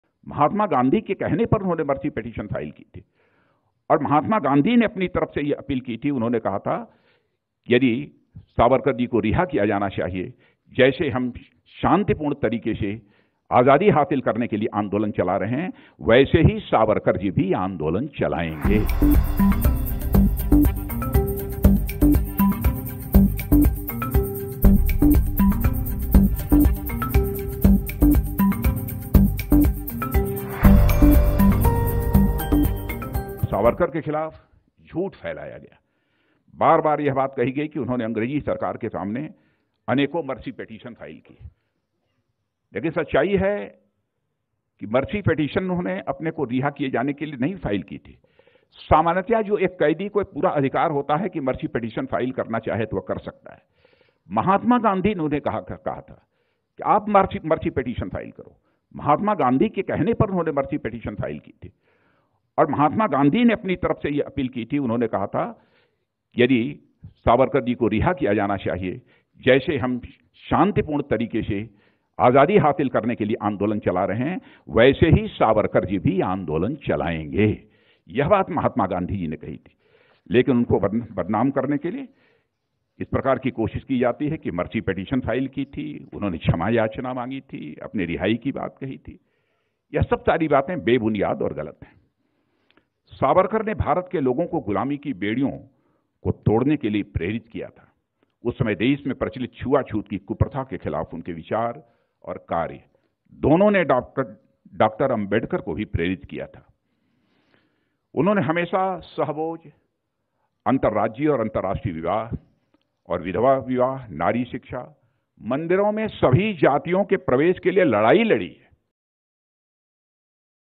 Rajnath Singh : Mahatma Gandhi చెప్పడం వల్లే Savarkar ఆ నిర్ణయం తీసుకున్నారు |BBC Telugu
భారత స్వతంత్ర ఉద్యమంలో సావర్కర్ క్షమాభిక్ష పిటిషన్ మీద ఎన్నో వాదనలు, విమర్శలు ఉన్నాయి. అవన్నీ అవాస్తవాలని కొట్టిపారేసిన రాజ్‌నాథ్ సింగ్, ఆ క్షమాభిక్ష నిర్ణయం వెనుక వాస్తవాల్ని చెప్పుకొచ్చారు.